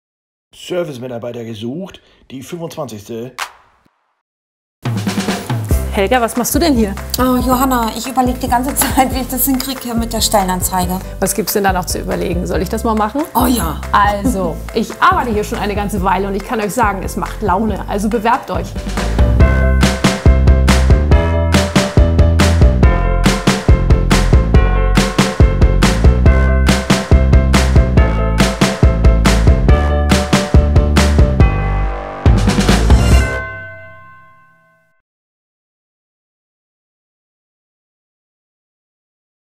Hotel Zweite Heimat sucht Service-Mitarbeiter!
Zweite Heimat St. Peter-Ording...Diesmal sind wir auf der Suche nach tollen Menschen, die unser Servive Team im Esszimmer verstärken wollen! Helga und Johanna haben sich spontan überlegt, wie wir euch am besten erreichen können. uns erreicht ihr am besten - Wir sehen uns hoffentlich bald in St. Peter-Ording!